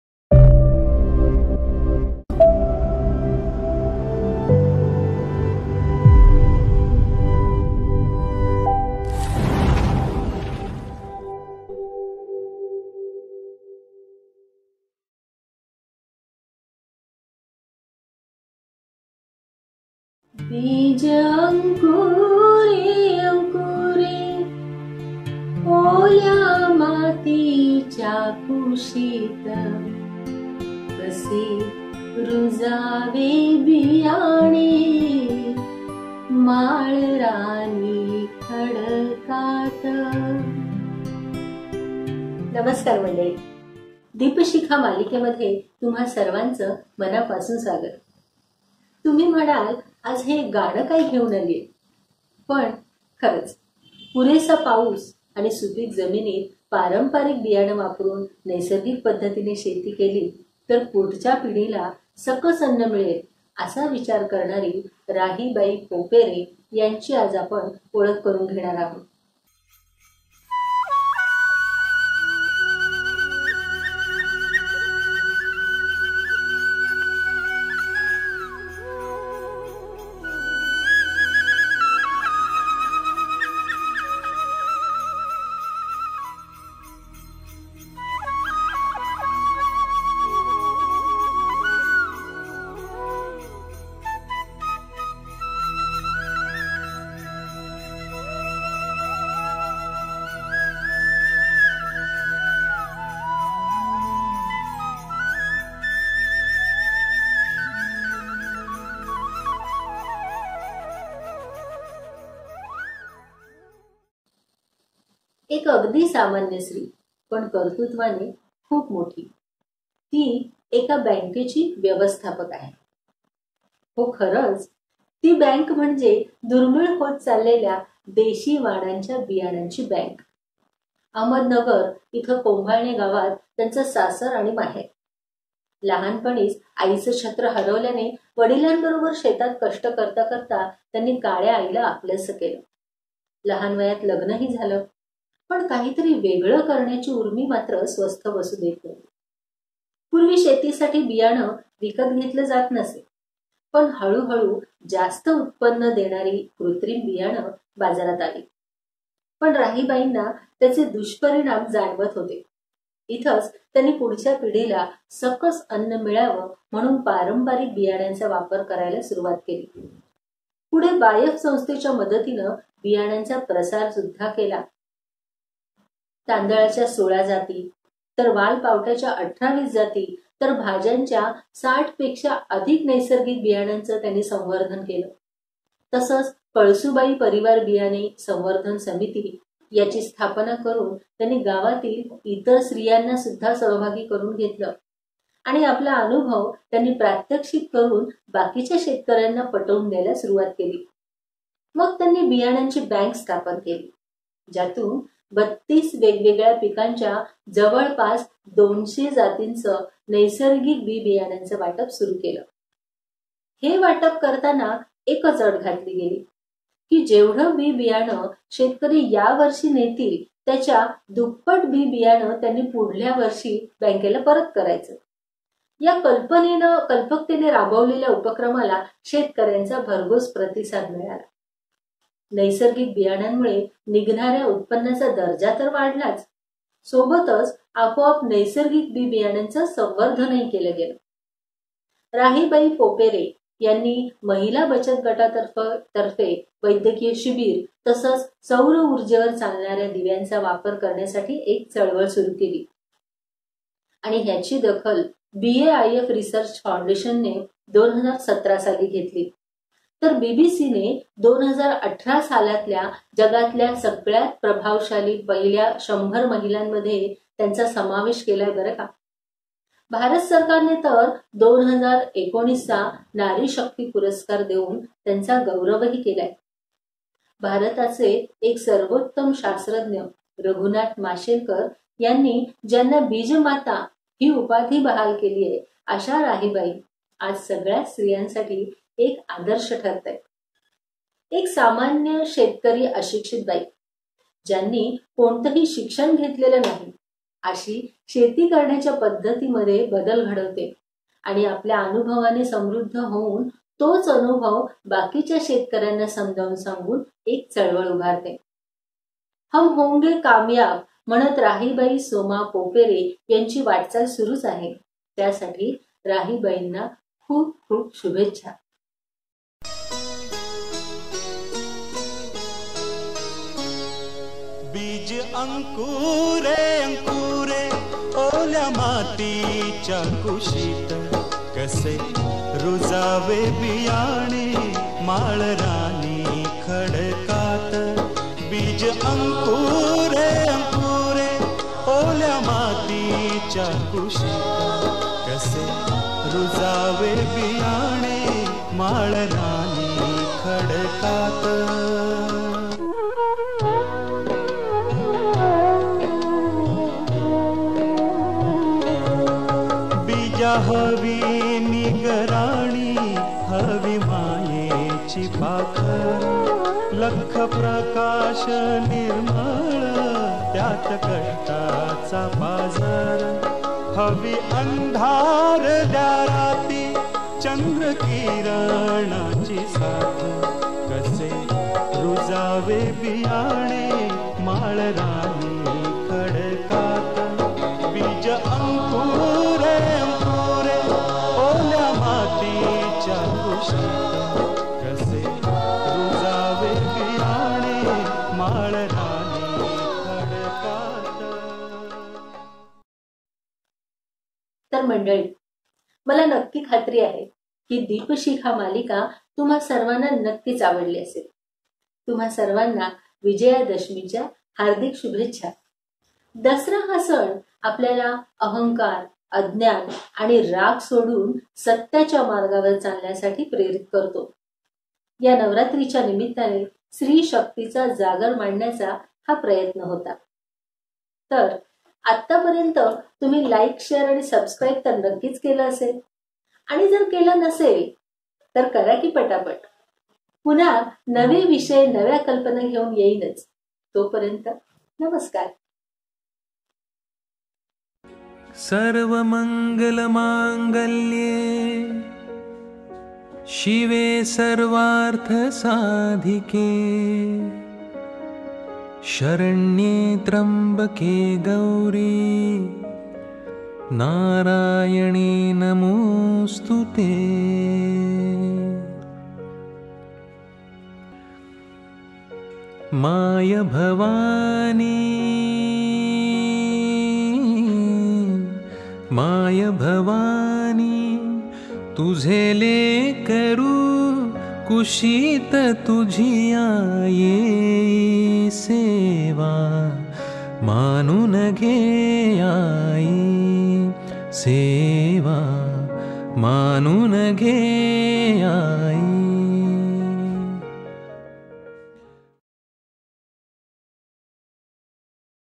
राहीबाई पोपेरे (Rahibai Soma Popere) - दीपशिखा भाग ९
दीपशिखा - भाग ९

आजची दीपशिखा : राहीबाई पोपेरे

सादरकर्ती : विभावरी खरडे

माहिती स्रोतः आंतरजालाहून

विशेष सहकार्य : विनोद देसाई